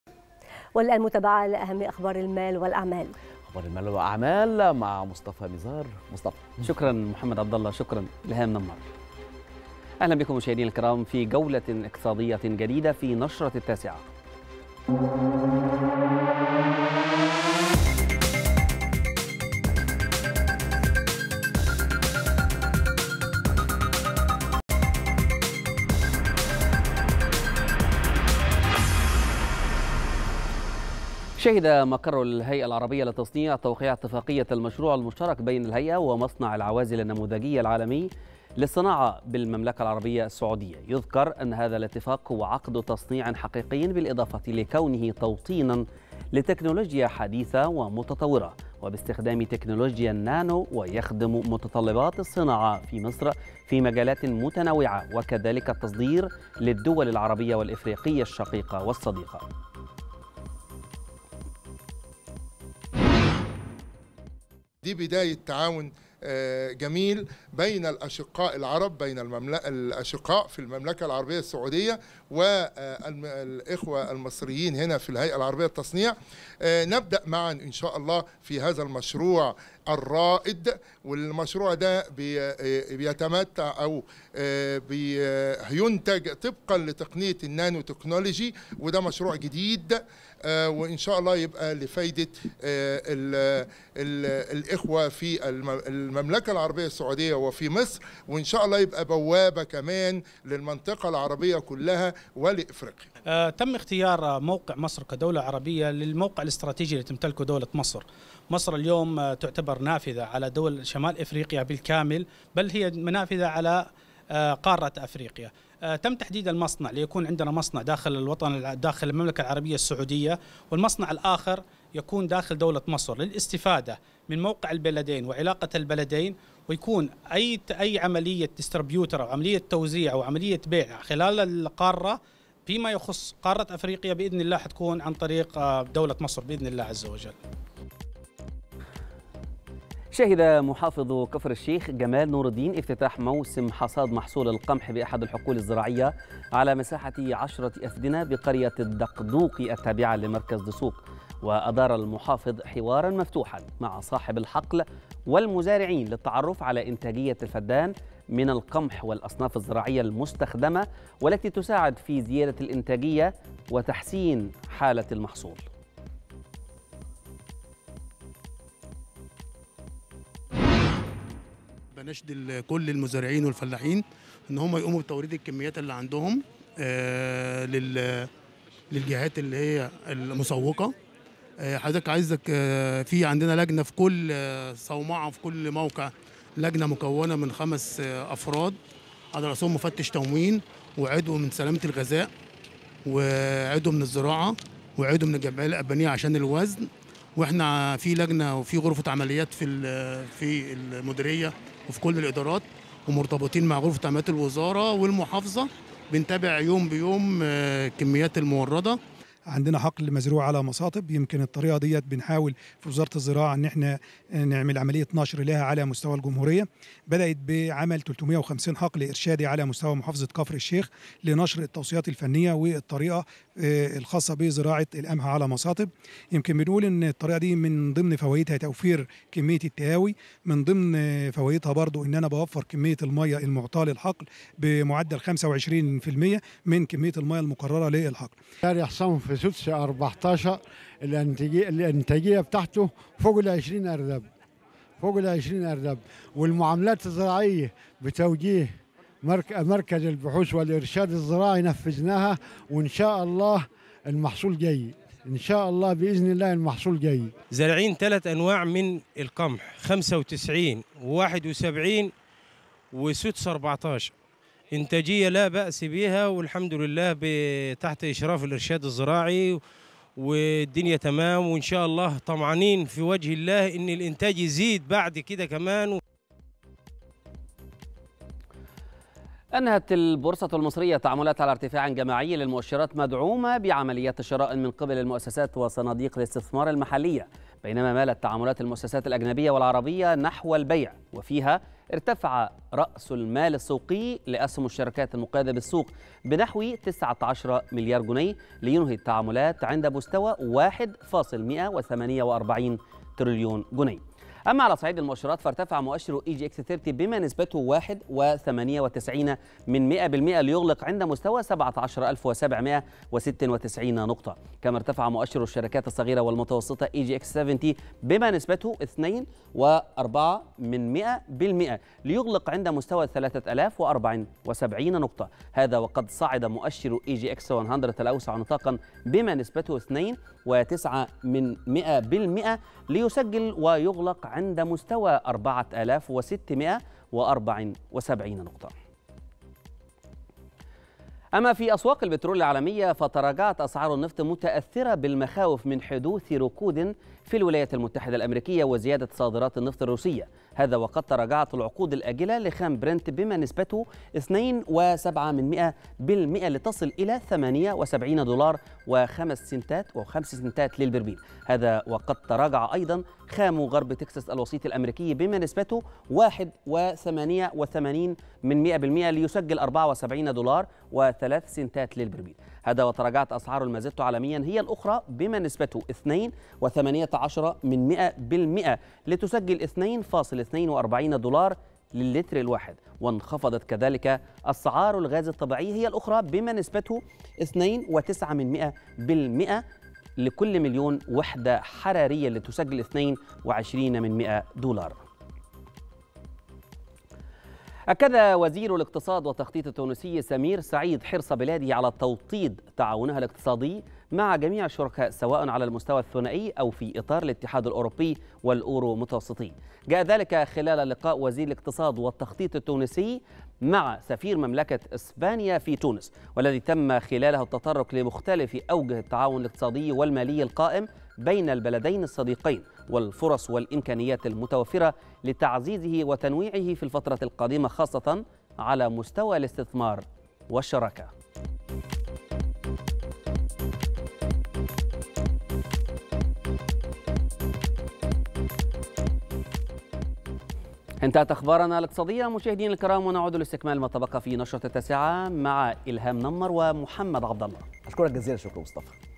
أهم أخبار النشرة الاقتصادية وأبرزها"الهيئة العربية للتصنيع توقع اتفاقية تعاون مع مصنع عوازل السعودي"
نشرة التاسعة | أهم أخبار النشرة الاقتصادية وأبرزها .. " الهيئة العربية للتصنيع توقع اتفاقية تعاون مع مصنع عوازل السعودي "